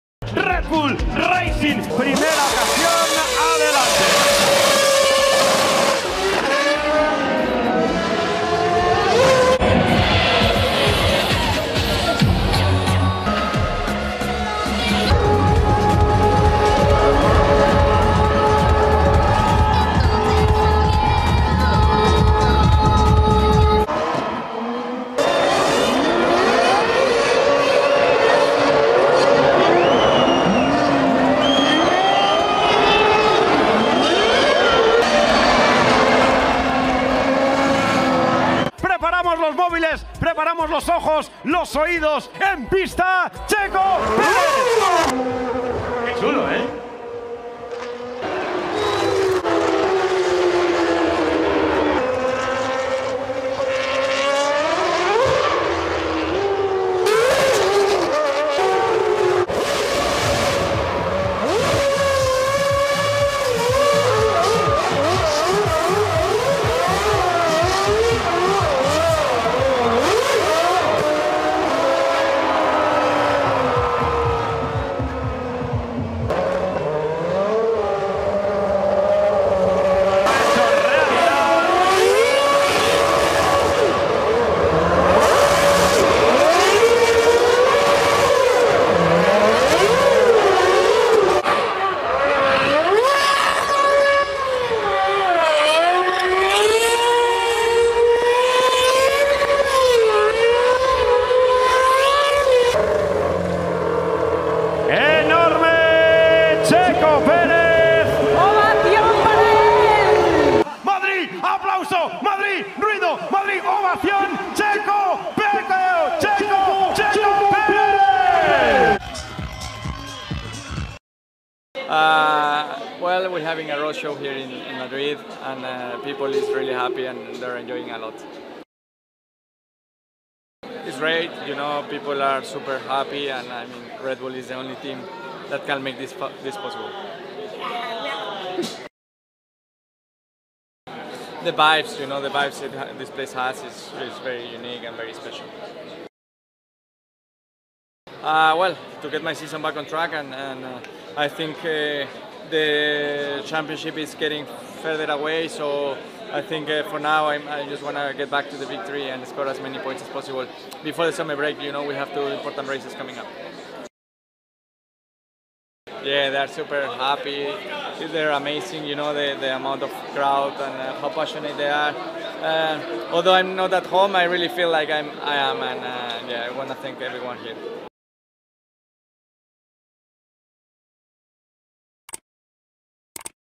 Red Bull star Perez takes F1 car to streets of Madrid 2023. [HD]
Red Bull star Sergio Perez took one of the team's Formula 1 cars to the streets of the Spanish capital Madrid for a show event